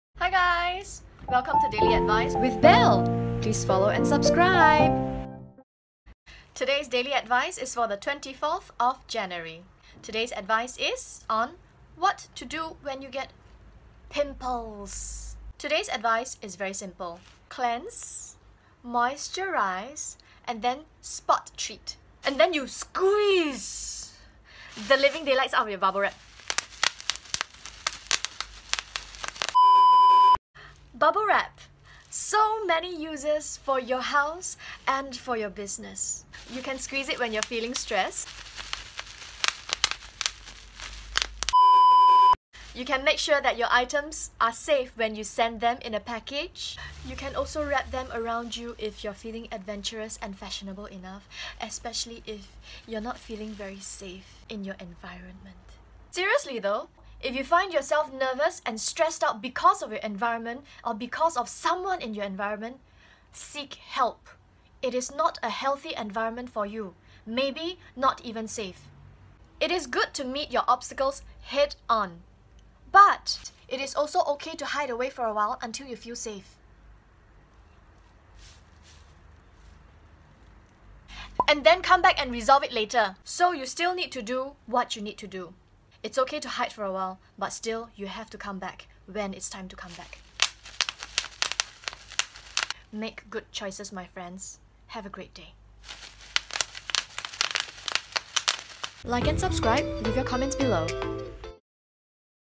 Daily Advice with Belle! 24th Jan: what to do when you get pimples due to distress 贝乐小贴士：因压力长痘痘该怎么办？
Today's dating advice is for the 24th of January.

Daily Advice with Belle! 24th Jan: what to do when you get pimples
贝乐小贴士：因为压力长痘痘该怎么办？

Pimples could be a symptom of bodily imbalances, lack of sleep and stress. Sooo what to do if you are often distressed?
长痘痘可能是身体里的不平衡，睡眠不足或者压力所导致。那么如果总是觉得有压力的话，该怎么办？

*note: The video clip mentions that you ought to go back “to do what you need to do”. However, if doing so could cause bodily harm or even result in death, it is better to call the police for help.
*注：视频里说到要回去面对：如果是会导致生命危险的话，就不要回去了，直接报警比较实际。

Belle
贝乐

My Channel's Keywords! Wisdom | Singapore | Southeast Asia | Wisdom | Joy | Business | Beauty | Success | Expertise | Professional | Breakthrough |

Transcript

Today's advice is on what to do when you get pimples
今天的贴士是长痘痘该怎么办？

Today‘s advice is very simple
今天的贴士非常简单

Cleanse, moisturise and then spot treat
清洗，滋润然后点式治疗

and then you squeeze the living daylights out of your bubble wrap
然后就死命地挤～～～包装泡沫

bubble wrap
哎 包装泡沫

So many uses for your house
家里那么多用途

and for your business
在办公室也是很多用处

You can squeeze it when you're feeling stressed
你感到压力的时候可以挤压

You can make sure that your items are safe when you send them in a package
在寄东西时，它会保护着东西不破损

You can also wrap them around you
你也可以把它把自己包起来

if you‘re feeling adventurous and fashionable enough
如果你想尝试走在时尚尖峰的话

especially if you're not feeling very safe in your environment
尤其是如果觉得自己觉身在不安全的地方

Seriously though, if you find yourself nervous and
说真的，如果你觉得紧张和

stressed out because of your environment or
有压迫感，不管是因为环境

because of someone in your environment
还是某某人

seek help
必定要求救哦

It is not a healthy environment for you, maybe not even safe
那个环境可能不健康，甚至不安全

seek help, especially if it's not safe for you
请求救，尤其是威脅到你的安全的话

It is good to meet your obstacles head on
虽然见山爬山，见海下海是件好事

But it is also okay to hide away for a while, until you feel safe
可是如果你觉得躲开比较好的话也没错

and then come back and resolve it later
你可以之后再回来解决那件事

So you still need to do what you need to do
所以还是得做你得做的事的

It’s okay to hide for a while, but still, you’ll have to come back when it's time to come back
闪避不要紧，只要你记得回来面对、解决状况。

make good choices my friends
做好的选择哦

祝你今天顺心如意